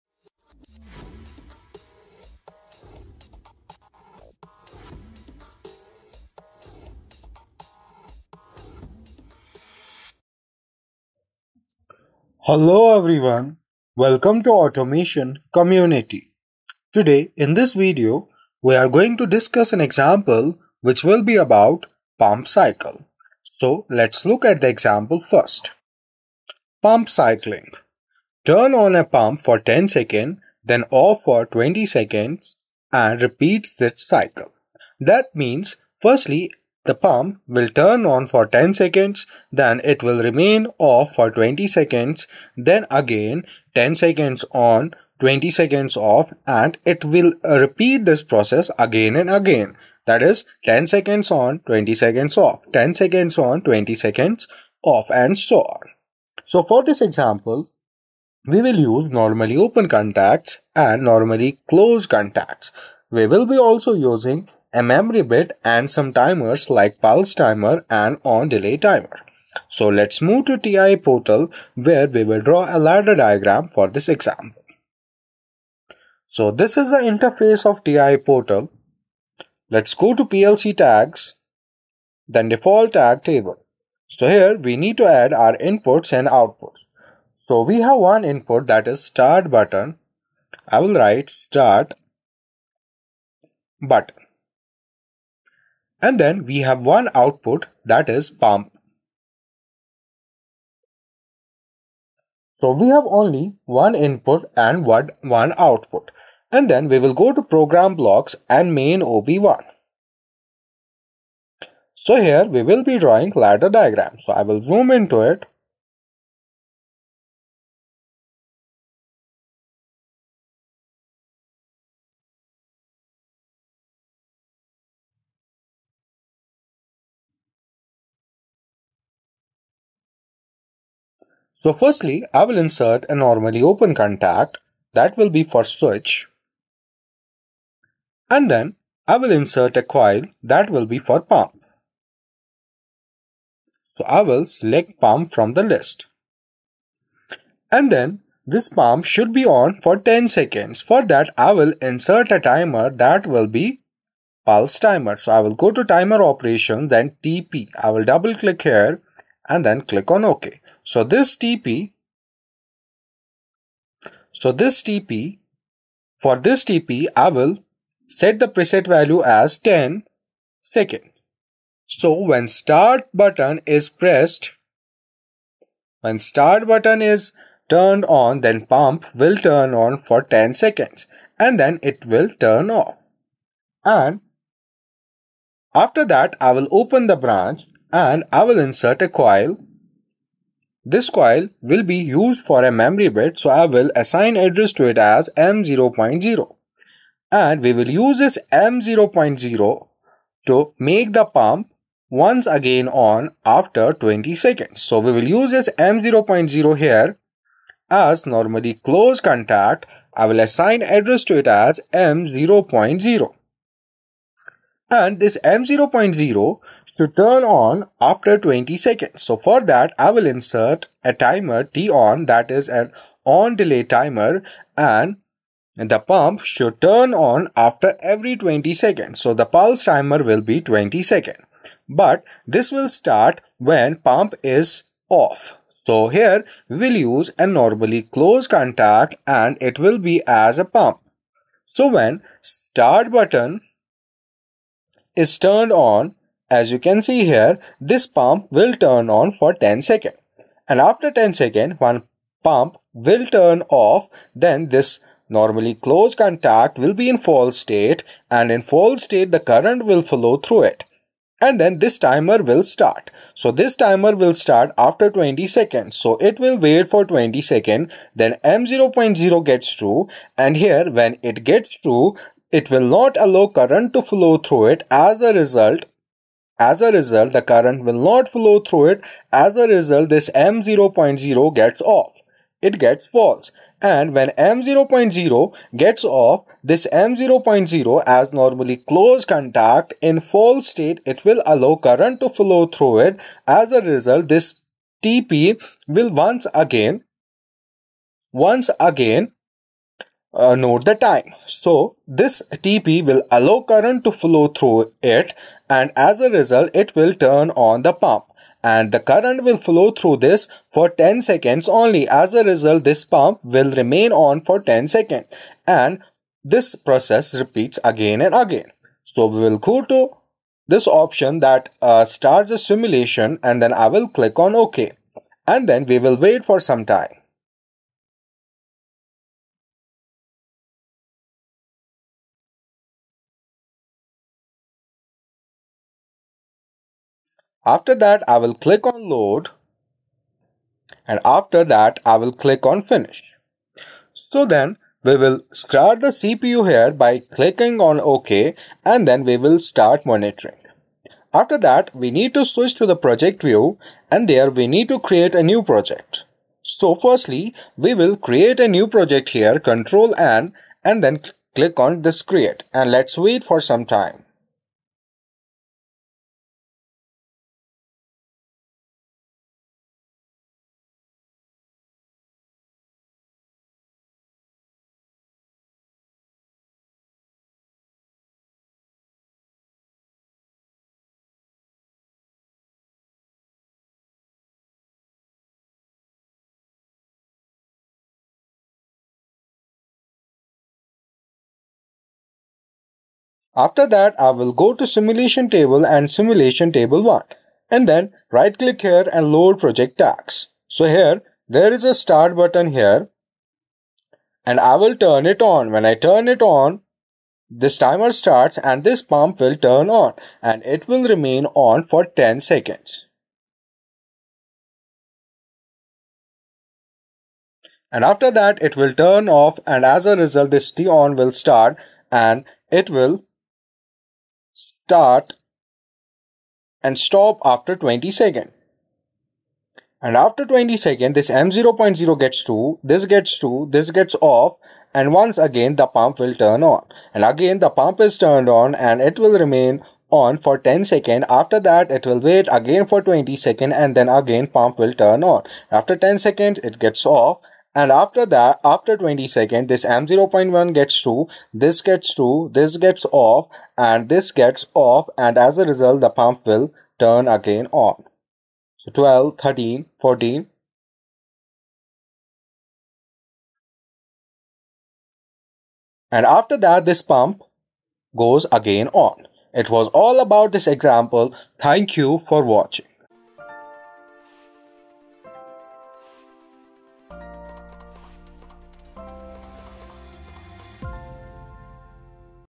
Siemens PLC: ON for 10 Seconds, OFF for 20 Seconds, Repeat Cycle
Learn how to program a Siemens PLC for a 10-second ON and 20-second OFF with a repeat cycle in industrial systems.